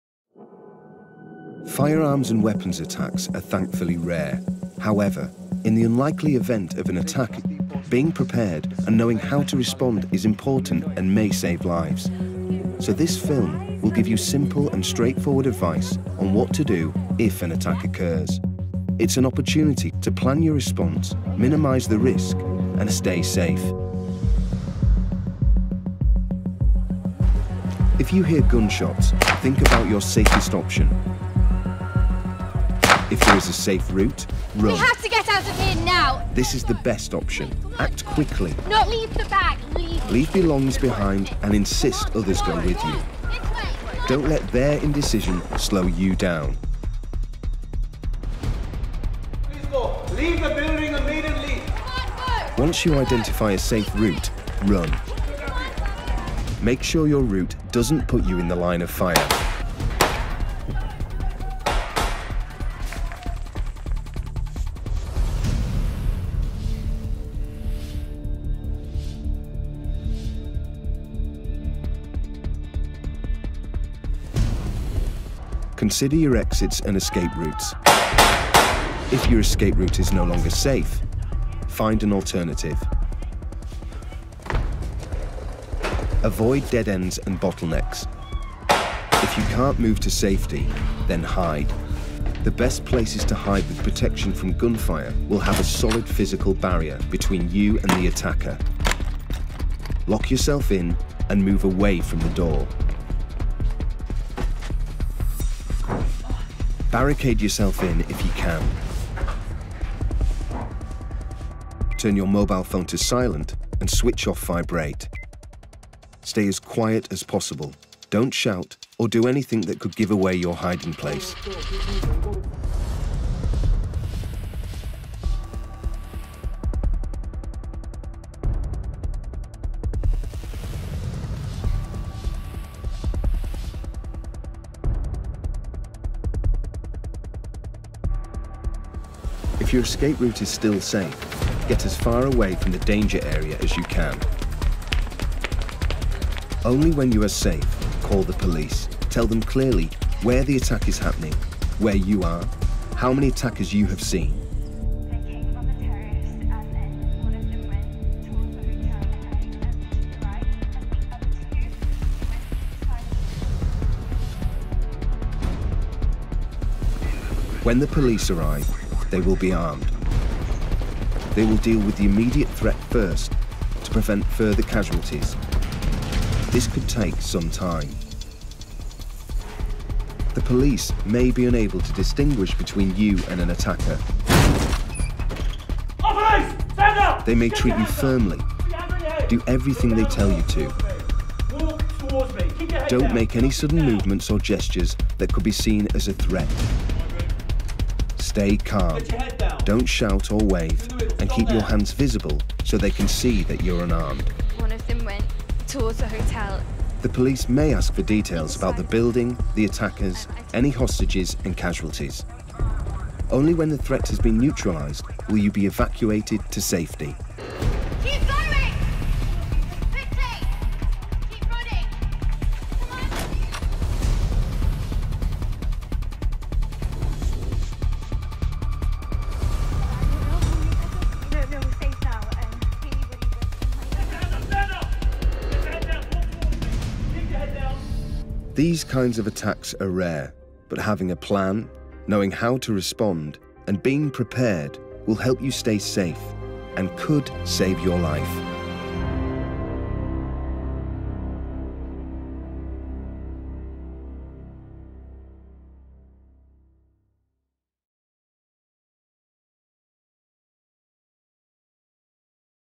Run Hide Tell Stay Safe in the event of a terrorist attack
Saiba o que fazer caso se encontre envolvido num cenário de ataque com armas de fogo.